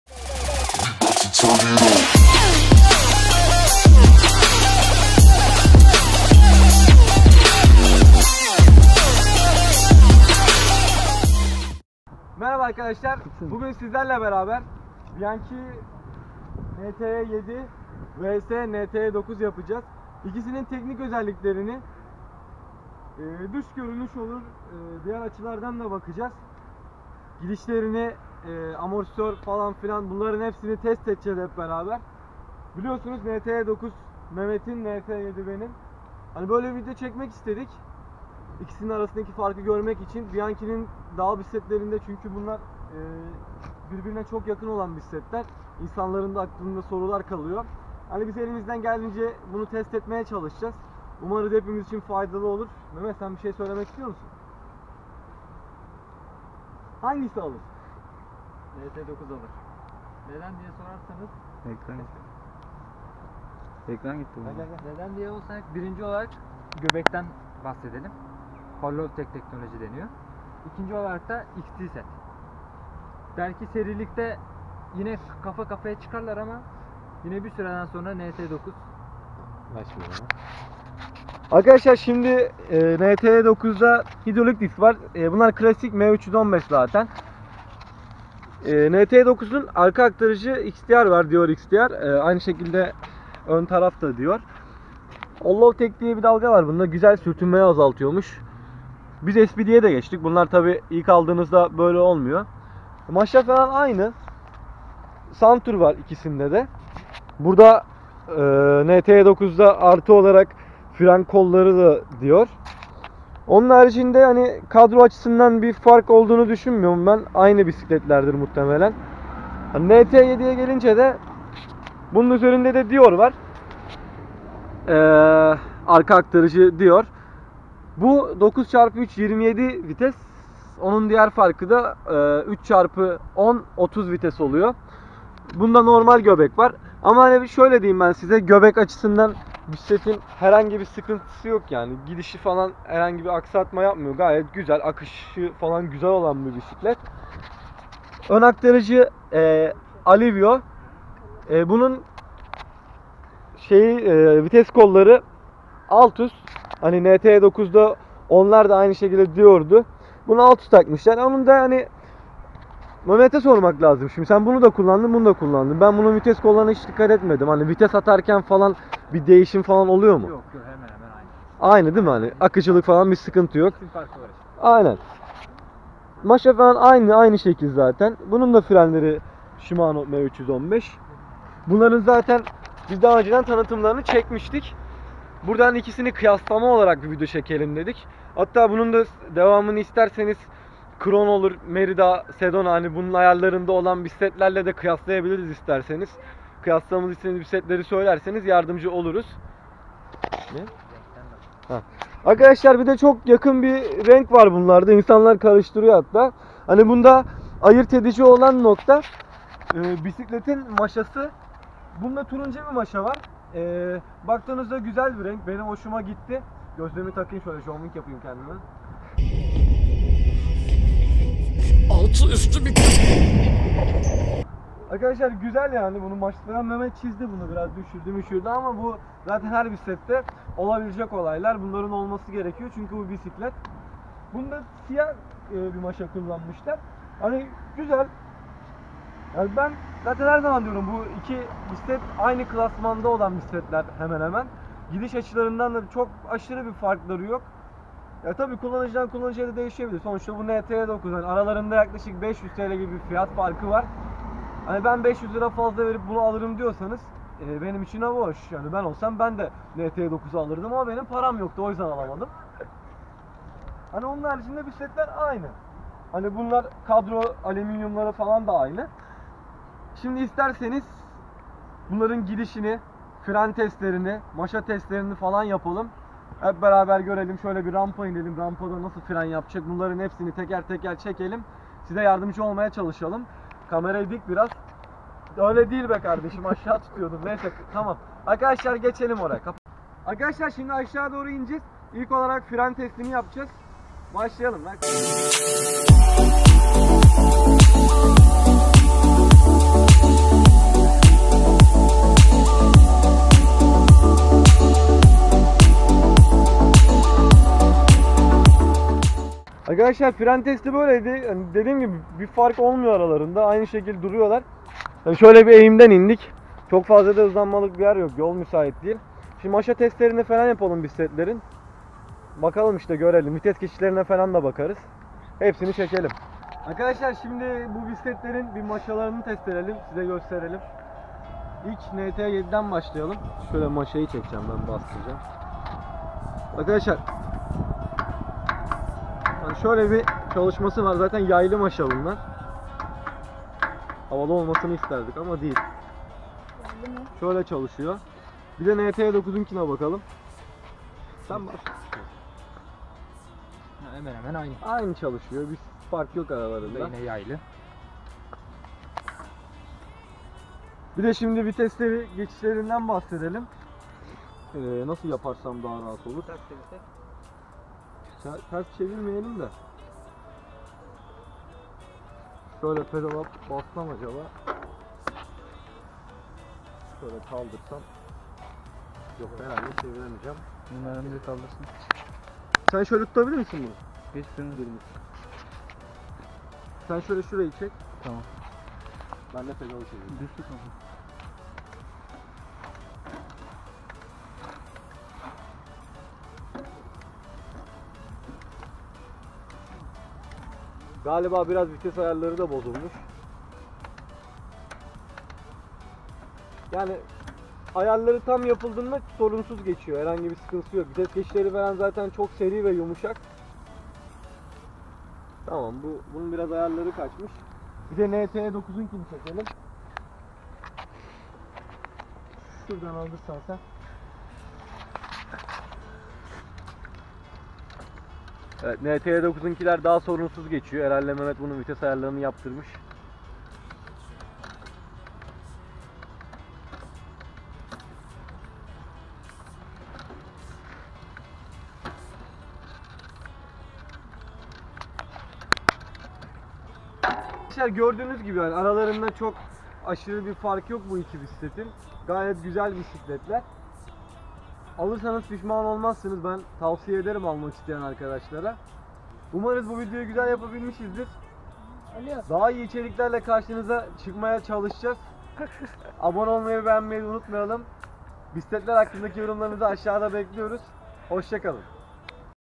BİANCHİ NTH 9 - VS - BİANCHİ NTH 7 ( YouTube' de İLK!! ) - BSİKOVLOG
MERHABA ARKADAŞLAR BUGÜN  BİANCHİ NTH 9 VS NTH 7  YAPTIK
İKİSİNİ TEKNİK AÇILARDAN VE SÜRÜŞ TESTLERİYLE KIYASLADIK
UMARIZ FAYDALI OLUR:)